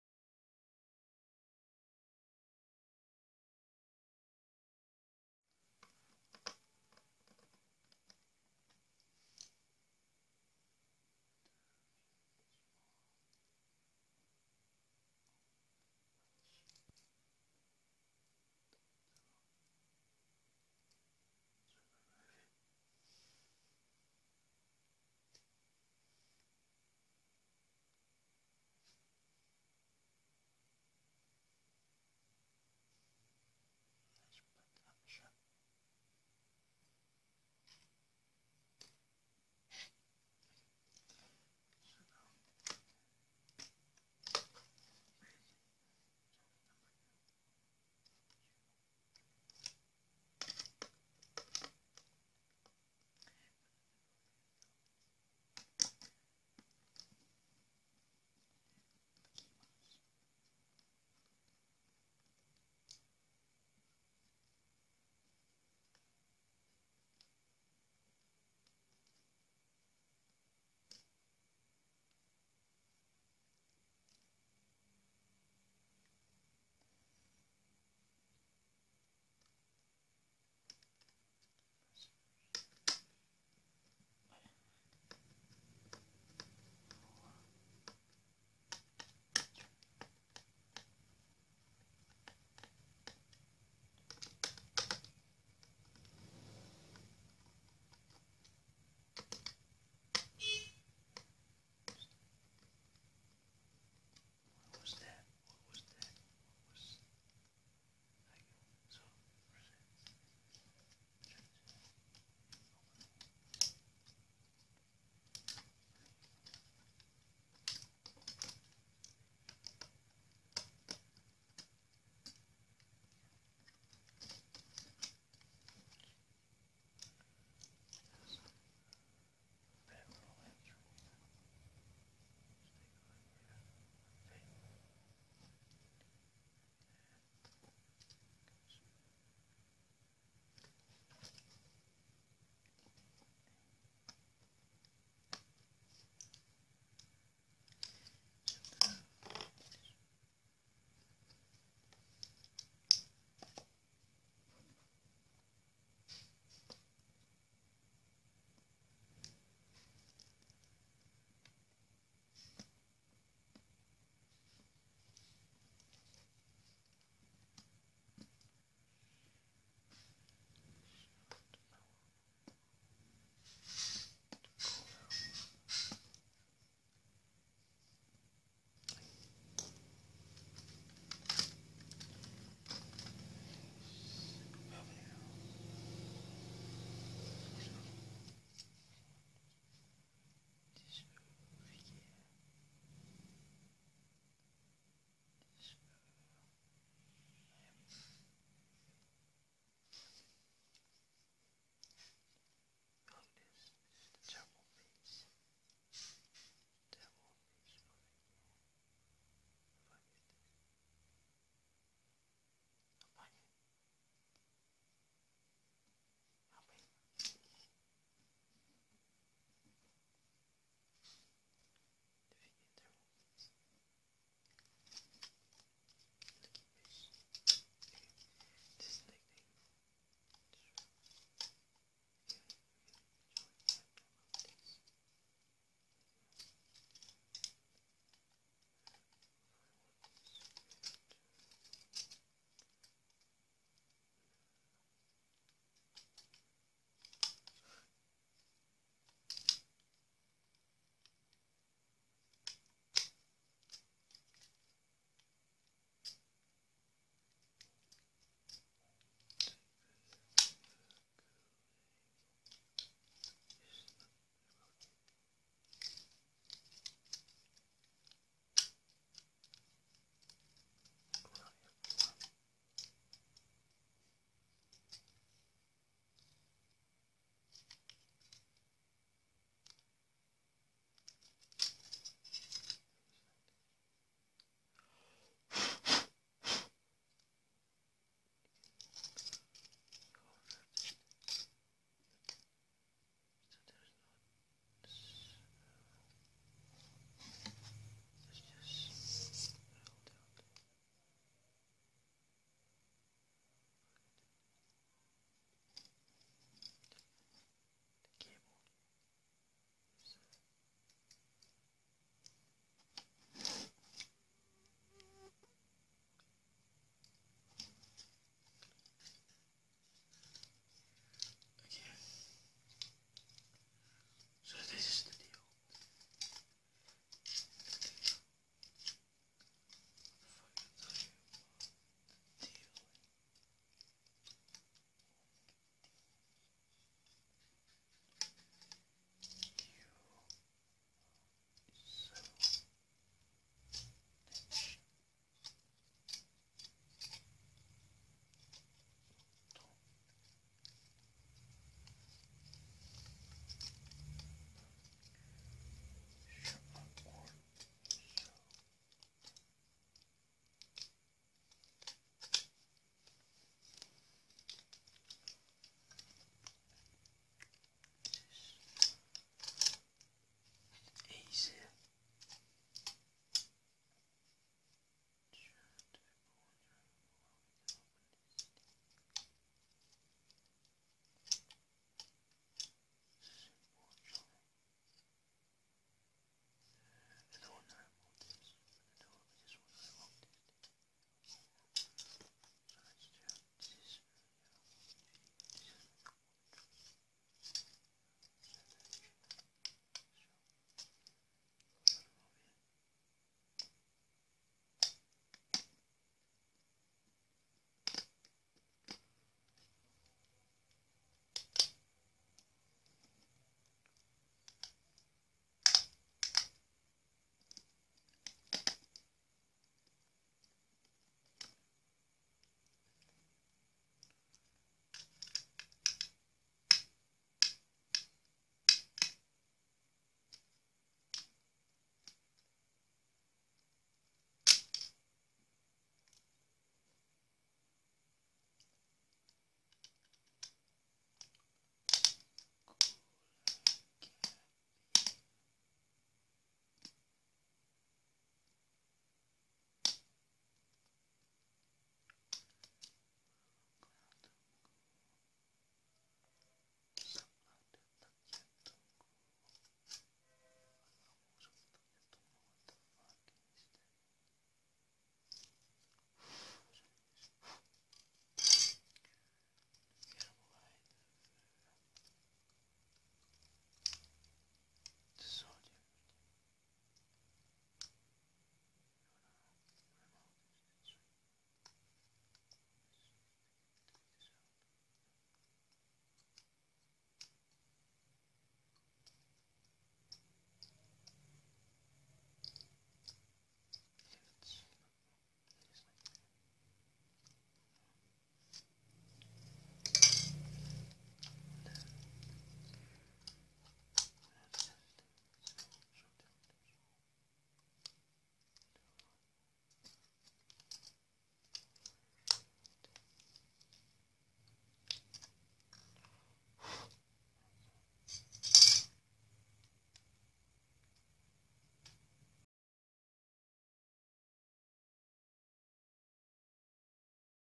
Perawatan Acer Aspire 4530 #4
membuka kipas prosesor